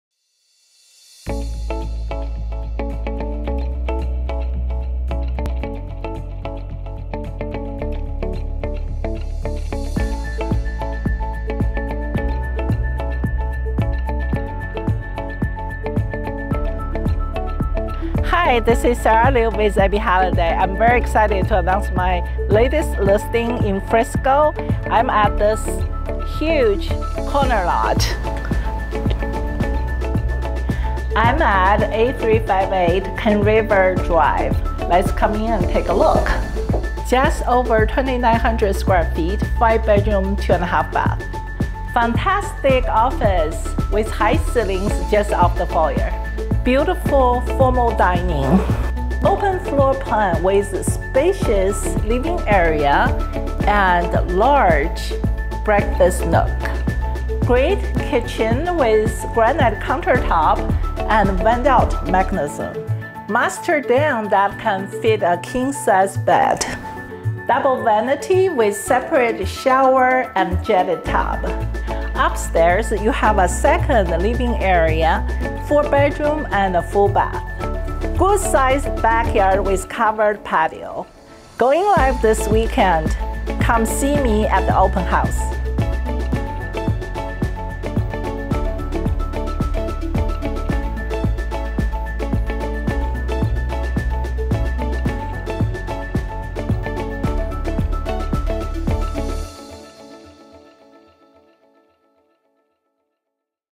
Elegant and Spacious Home on a Corner Lot Frisco, TX Ebby Halliday, REALTORS
Introducing an elegant and spacious home located on a large corner lot, designed to cater to the needs and desires of every family member. As you step inside, you'll be greeted by the office with its high ceilings and abundant natural light, providing the perfect space for work or study. The open floor plan seamlessly connects the generous living area to the oversized eat-in kitchen, complete with granite countertops, ample storage, and a vent mechanism leading to the outdoors. Hosting guests is effortless with the addition of a lovely formal dining room.

The thoughtfully designed split bedroom floor plan ensures privacy and convenience. Downstairs, the primary retreat awaits, featuring an en-suite bathroom with double vanities, a separate shower, and a luxurious jetted tub for ultimate relaxation. Upstairs, you'll find four additional bedrooms, along with an open game room, creating a versatile space for leisure and entertainment. One of the secondary bedrooms offers the flexibility to be transformed into an additional office or hobby room, catering to your specific needs.

The backyard is a true haven, featuring a covered patio and a designated play area for children. Additionally, there is a garden bed perfect for cultivating vibrant flowers and enhancing the outdoor ambiance. Situated in a prime location close to the heart of Frisco, this home offers easy access to a wide array of dining, shopping, and entertainment options. Within just 15 minutes, you'll reach the vibrant Legacy West area, renowned for its upscale offerings.

Not to be overlooked, this home is nestled within the desirable Frisco school district, ensuring access to exceptional educational opportunities for your family. Don't miss out on the chance to make this extraordinary property your own. Schedule a viewing today and discover the possibilities!